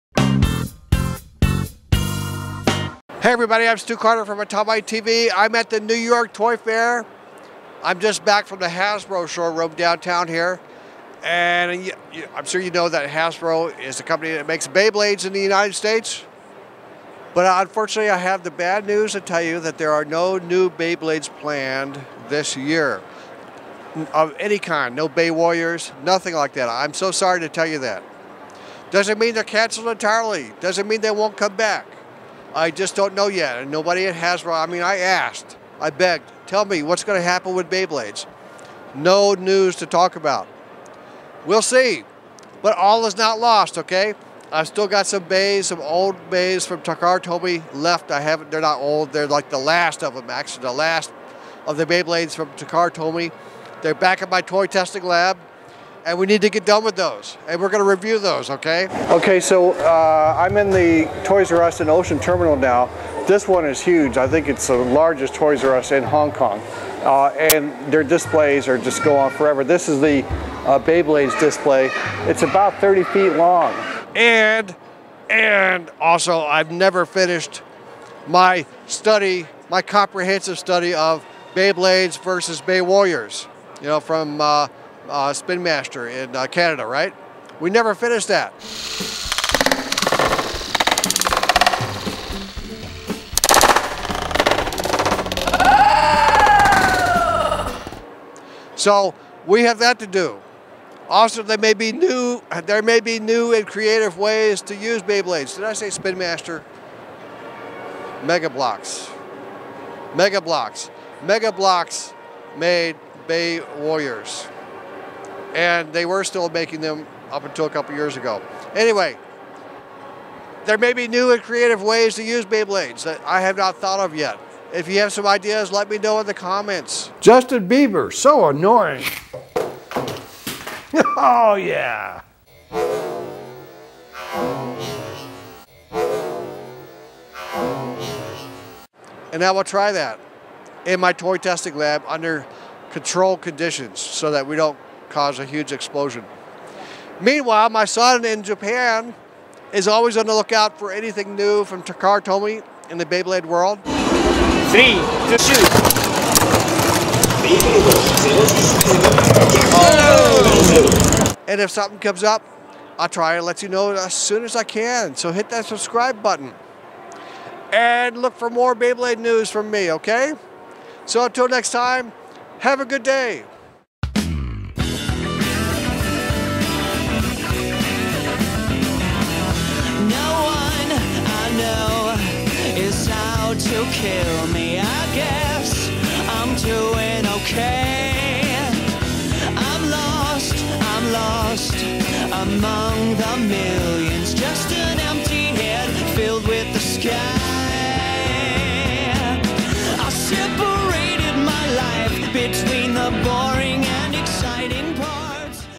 Beyblades Tops Report New York Toy Fair 2015
Beyblades report from the New York Toy Fair, February 2015.

Look for future reports on Beyblade tops by Takara Tomy, and Battle Strikers vs Beyblades